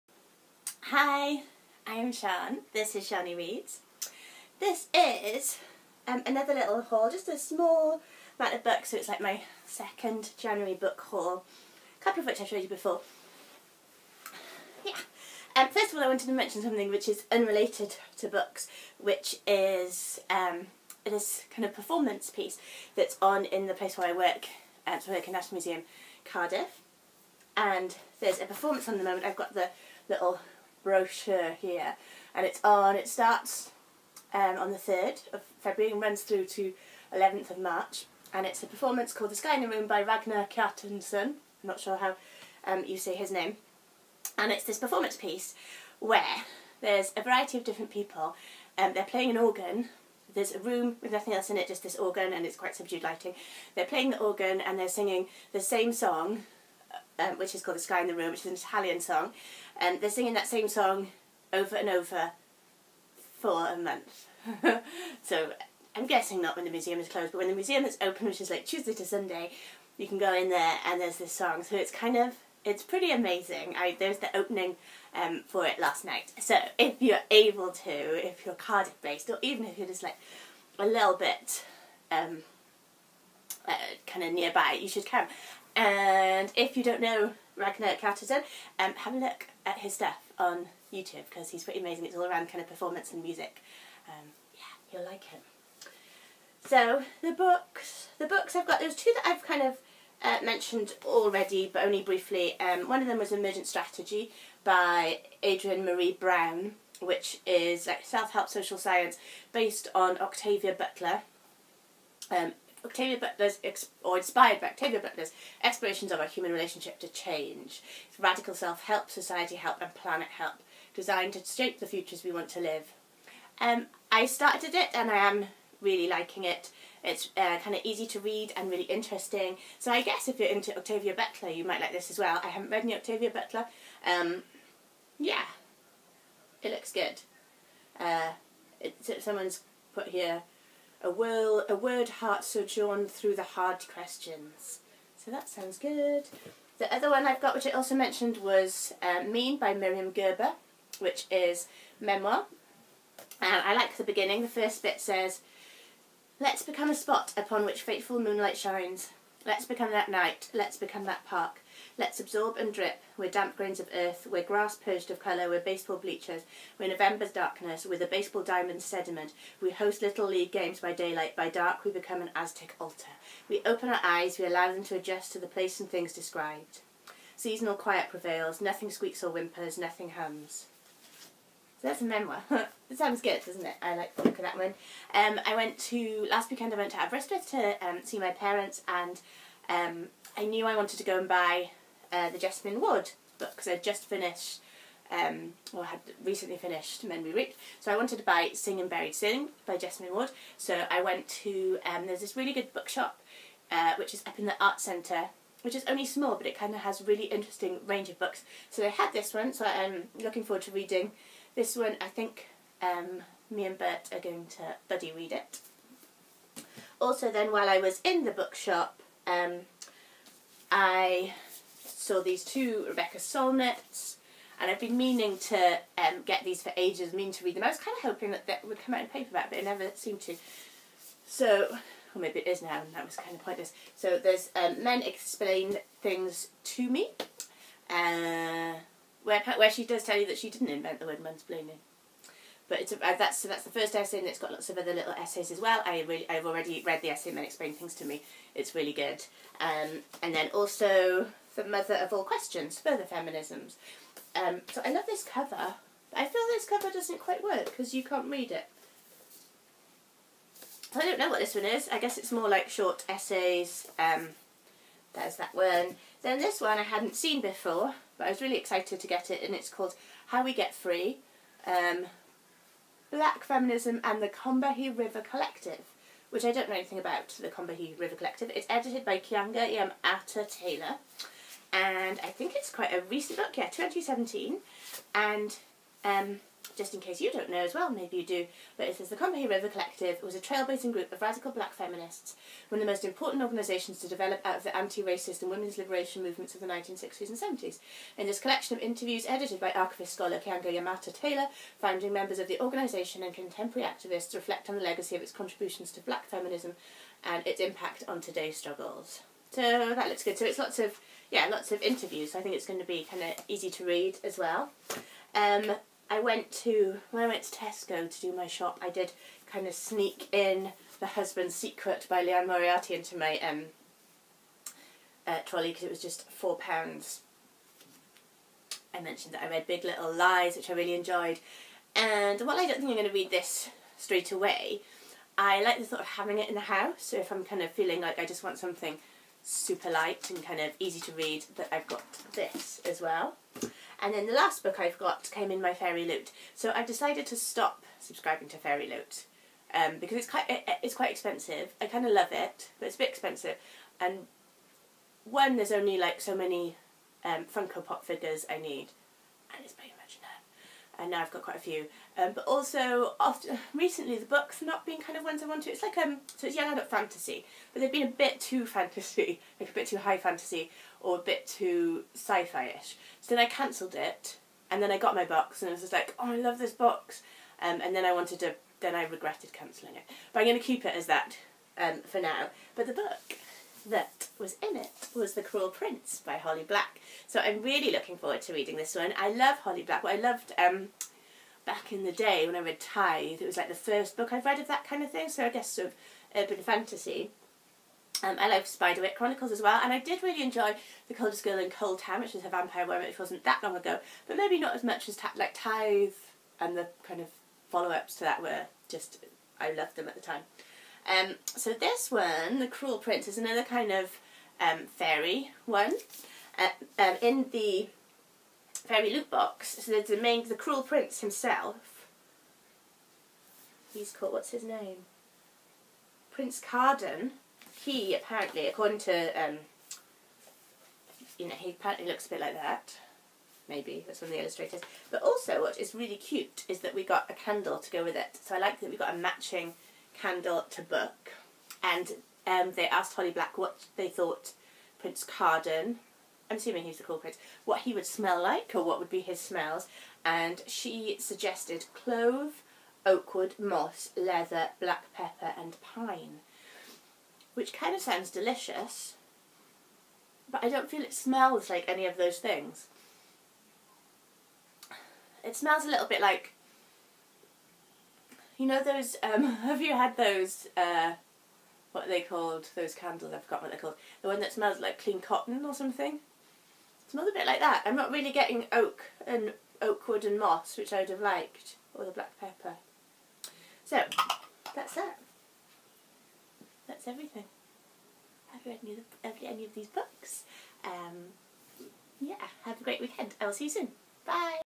January book haul part two
Some new books!
Emergent Strategy by Adrienne Maree Brown
Mean by Myriam Gurba
Sing Unburued Sing by Jesmyn Ward
Men Explain Things to me by Rebecca Solnit
The Mother of all questions by Rebecca Solnit
How We Get Free by Keeanga-Yamahta Taylor
The Husband's Secret by Liane Moriarty
The Cruel Prince by Holly Black